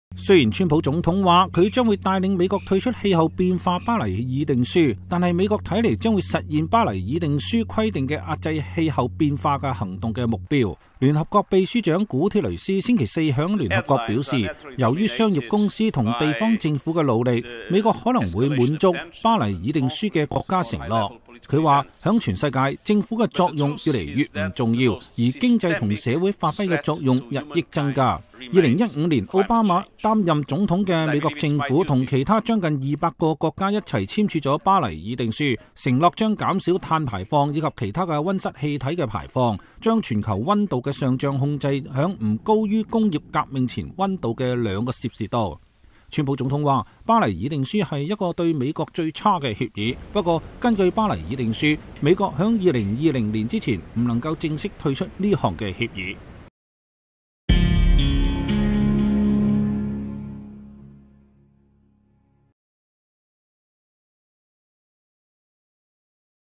聯合國稱美國將實現巴黎議定書規定目標
雖然川普總統說他將帶領美國退出氣候變化巴黎議定書，但是美國看來將實現巴黎議定書規定的遏制氣候變化行動的目標。
聯合國秘書長古鐵雷斯星期四在聯合國說，由於商業公司和地方政府的努力，美國可能會滿足巴黎議定書的國家承諾。他說：“在全世界，政府的作用越來越不重要，而經濟和社會發揮的作用日益增加。”